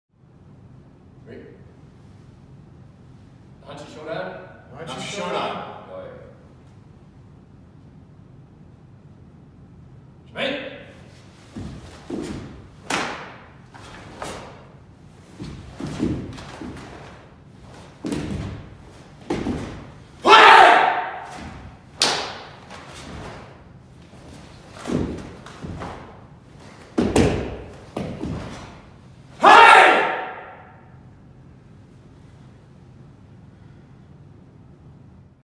Naihanchi Shodan - Matsubayashi-Ryu
Naihanchi Shodan performed by:
Left: Jason Pennell
Center: Daniel O'Brien
Right: Gary Christensen